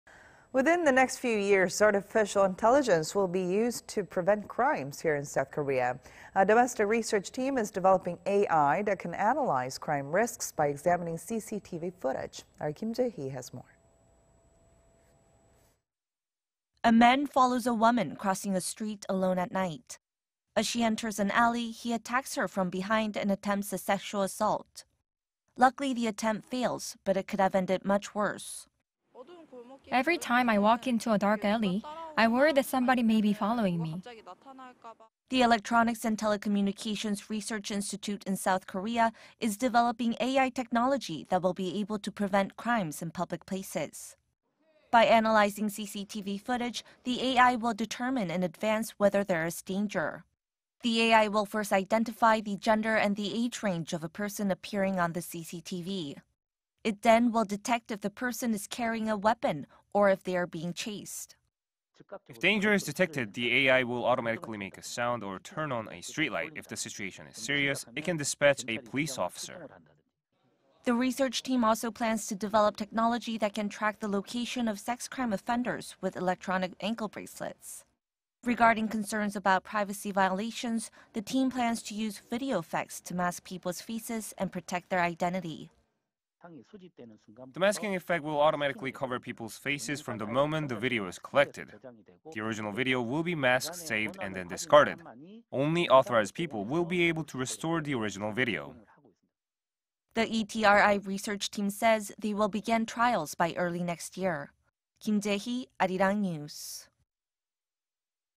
S. Korea to combine AI and CCTV systems to prevent crimes
강력 범죄 예측•대응하는 인공지능 나온다

Within the next few years, Artificial Intelligence will be used to prevent crimes here in South Korea.
A domestic research team is developing AI that can analyze crime risk by examining CCTV footage.
Kim Jae-hee has this report.
A man follows a woman crossing a street alone at night.
As she enters an alley, he attacks her from behind and attempts a sexual assault.
Luckily, the attempt fails,... but it could have ended much worse.
"Every time I walk into a dark alley, I worry that somebody may be following me."
The Electronics and Telecommunications Research Institute in South Korea,... is developing AI technology that will be able to prevent crimes in public places.
By analyzing CCTV footage, the AI will determine in advance whether there is danger.
The AI will first identify the gender and the age range of a person appearing on the CCTV.
It then will detect if the person is carrying a weapon, or if they are being chased.
"If danger is detected, the AI will automatically make a sound, or turn on a street light. If the situation is serious, it can dispatch a police officer."
The research team also plans to develop technology that can track the location of sex crime offenders with electronic ankle bracelets.
Regarding concerns about privacy violations, the team plans to use video effects to mask people's faces and protect their identity.
"The masking effect will automatically cover people's faces from the moment the video is collected. The original video will be masked, saved, and then discarded. Only authorized people will be able to restore the original video."
The ETRI research team says they will begin trials by early next year.
Kim Jae-hee, Arirang News.